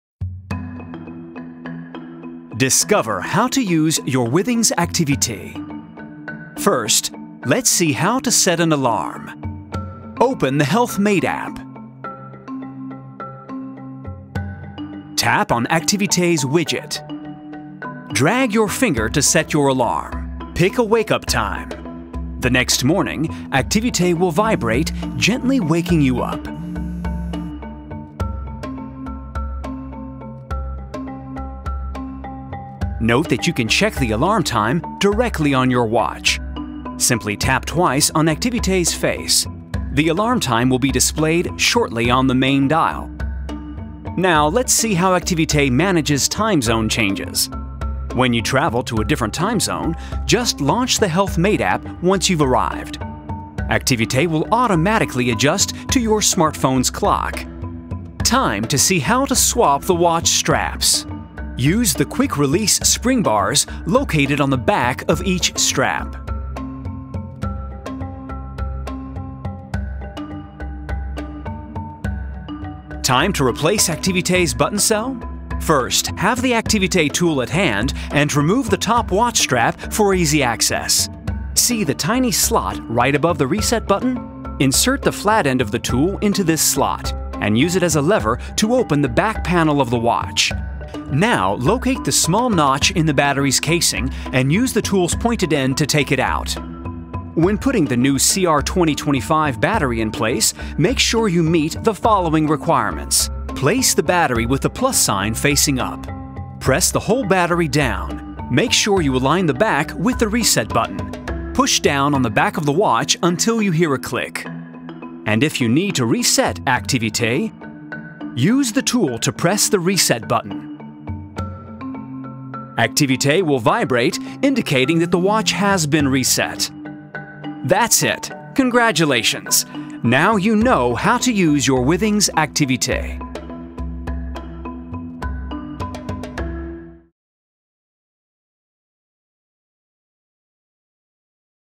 [EN] How to use your Withings Activité
Discover how to use your Withings Activité.
A new generation Swiss Made watch, combining time and activity tracking.
- Activity tracking (steps, elevation, distance, calories)
- Sleep tracking (light and deep sleep cycles, wake ups, total duration)
- Silent Alarm (wake up with a gentle vibration)
- Works on a button cell (no need to charge it)